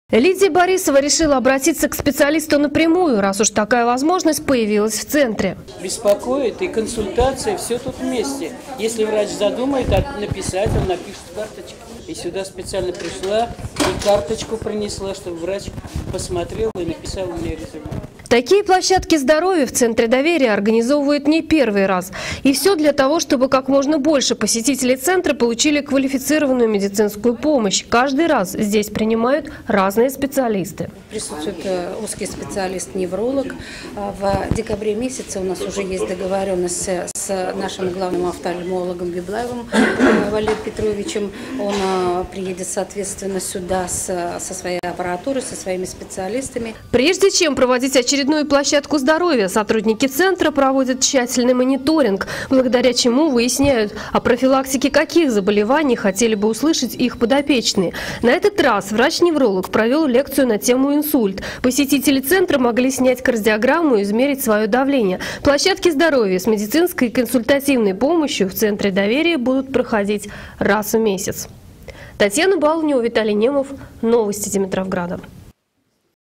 Площадки здоровья
Мероприятие организовали в четверг в центре "Доверие". Всего за пару часов несколько десятков пожилых людей смогли получить рекомендации медиков, сообщает